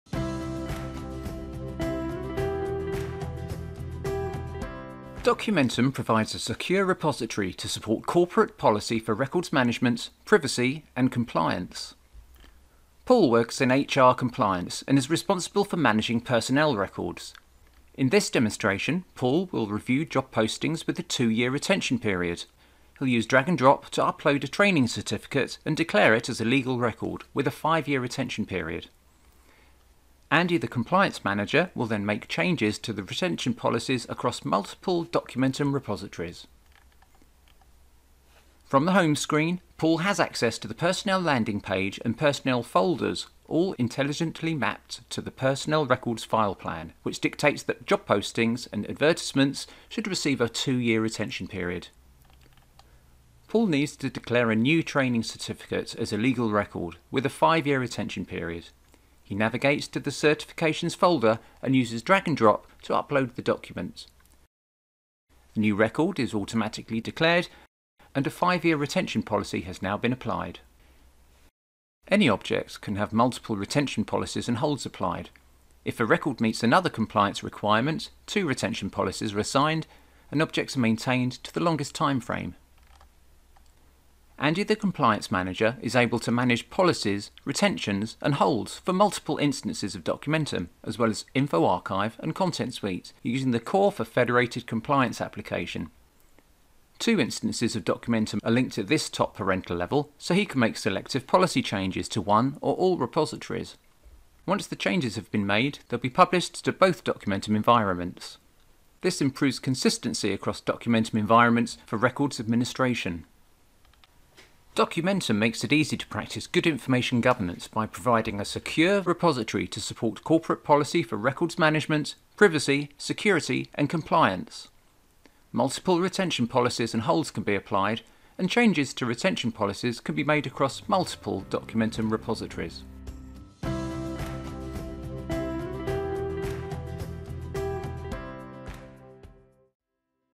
Managing retention policies in Documentum
Documentum makes it easy to practice good Information Governance.  Multiple retention policies and holds can be applied to content and changes to retention policies can be made across multiple Documentum repositories.